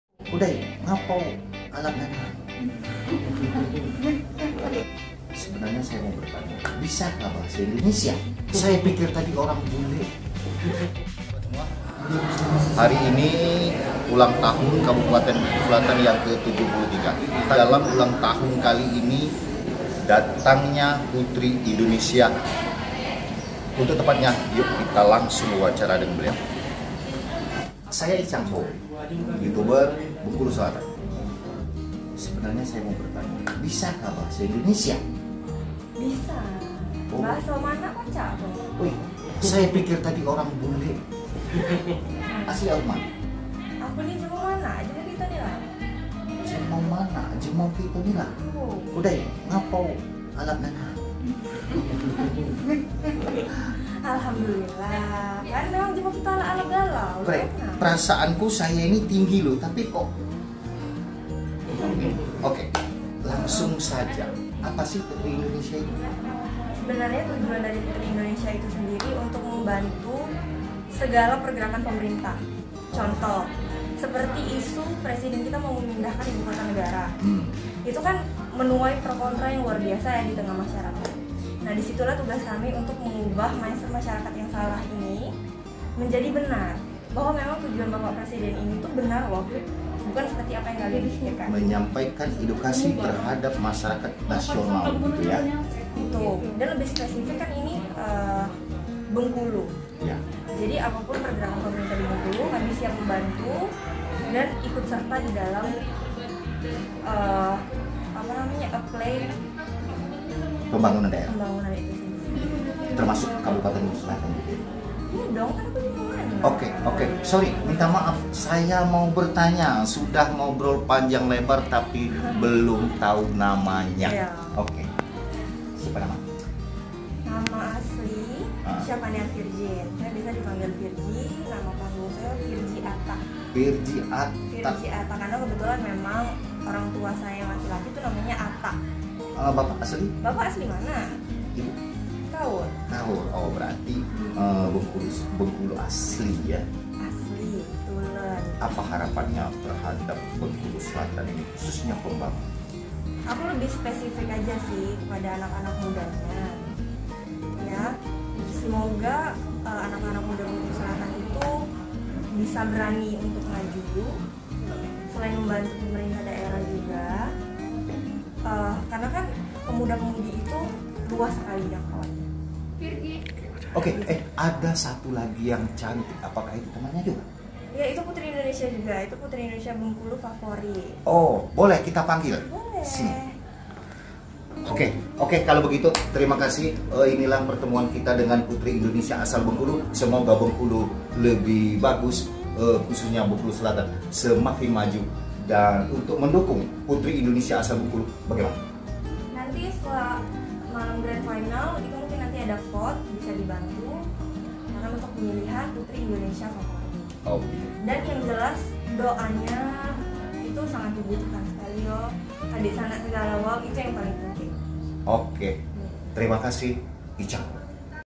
Inilah putri indonesia perwakilan bengkulu
inilah putri indonesia asal provinsi bengkulu ikut memeriahkan acara ulang tahun bengkulu selatan yang ke 73 tahun 2022.